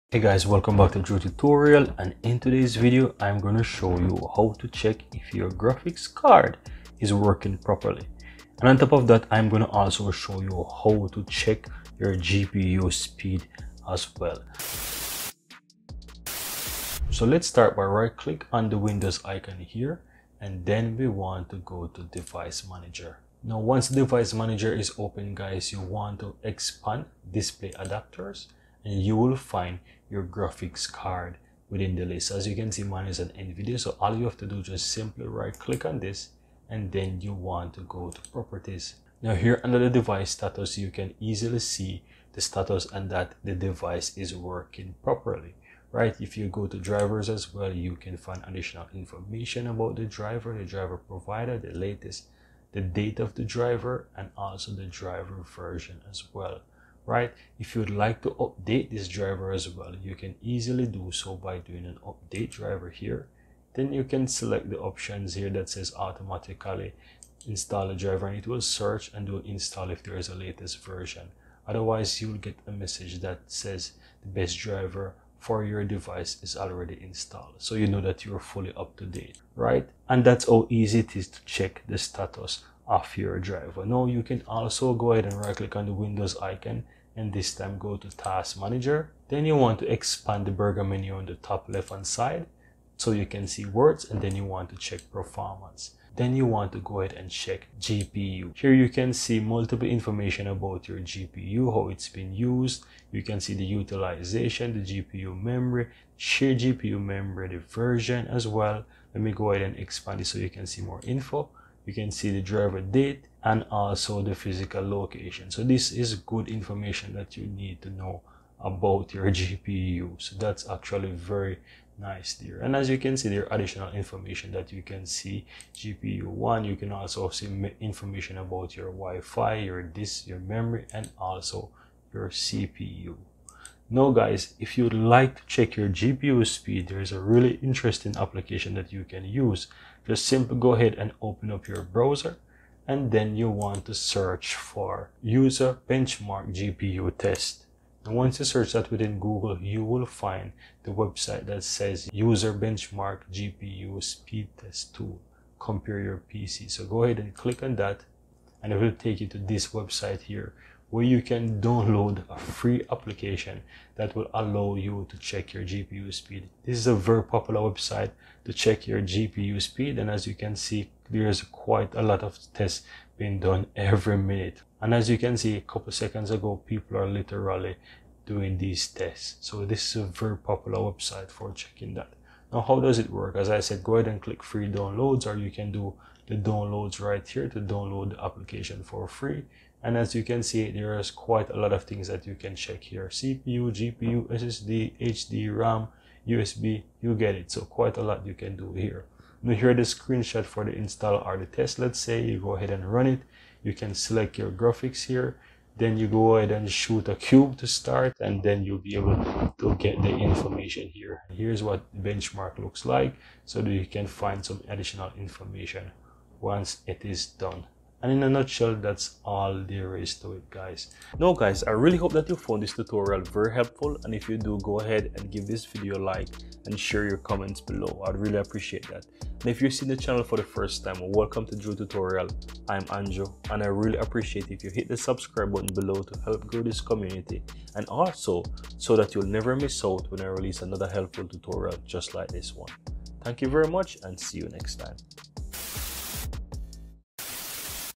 How to Check If Your Graphics Card is Working Properly | GPU Troubleshooting Guide
In this tutorial, we'll cover various methods to check the health and performance of your graphics card, helping you identify any potential issues that might be affecting your visual experience.

👉 Step 1: Device Manager Check
We'll begin by showing you how to verify that your graphics card is recognized by your system using the Device Manager. This quick check will ensure that your GPU is properly detected and installed.

👉 Step 2: GPU-Z Software Overview
Learn how to use GPU-Z, a powerful and free tool, to gather detailed information about your graphics card, including model, driver version, and more. This step is essential for understanding the specifications of your GPU.

👉 Step 3: Benchmarking with Heaven or FurMark
Discover how to use benchmarking tools like Heaven or FurMark to stress-test your graphics card and assess its performance under heavy loads. This step will help you identify any potential stability issues.

👉 Step 4: Checking Temperatures with MSI Afterburner
Overheating can lead to performance problems and damage your GPU in the long run. We'll guide you through using MSI Afterburner to monitor your graphics card's temperature and adjust fan settings for optimal cooling.

👉 Step 5: Monitoring Performance in Task Manager
Learn how to use the Task Manager to monitor GPU performance in real-time. This step is useful for identifying any spikes or abnormalities during regular usage.